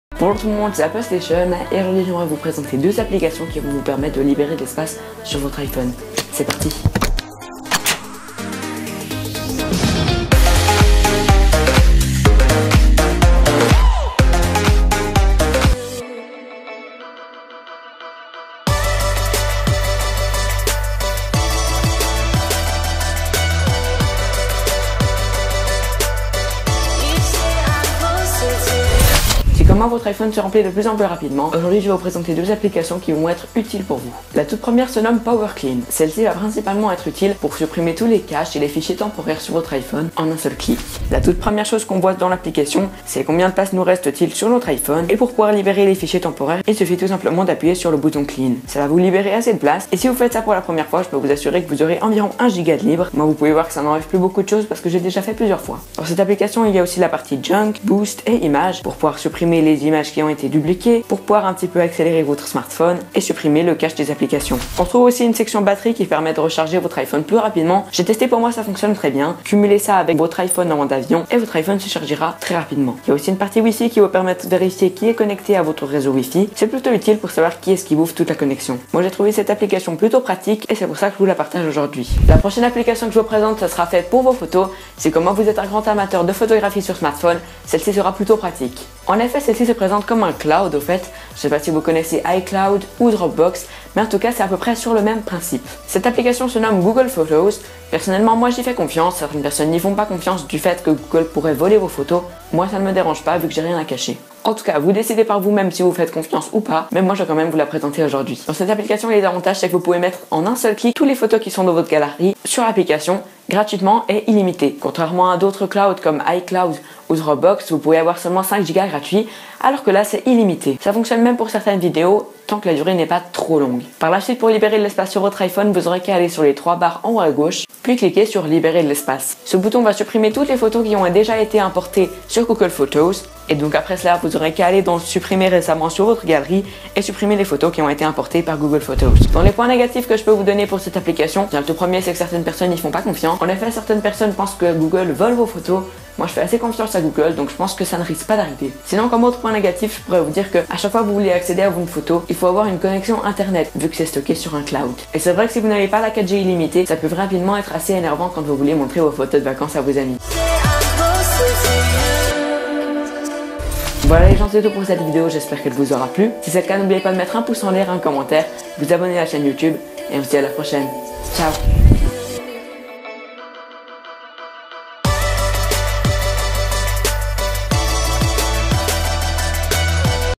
COMMENT LIBERER DE L'ESPACE SUR SON iPHONE ? [apps of the week #1]
Les deux applications présentés dans cette vidéo sont les suivantes :

-Power Clean
-Google Photos

Si vous lisez encore la description, mettez le mot ''utile'' en commentaire :p ! (je suis sur que personne ne lit les descriptions)

MES PRODUITS APPLE:
- iPhone 6 Gold 16gb
- iPad Mini 1 Blanc 16gb
- Apple Watch 1 Sport 42mm Noir 8gb
- MacBook Pro Retina 2014 256gb

Merci pour les 1100 Abonnés , c'est un truc de fou !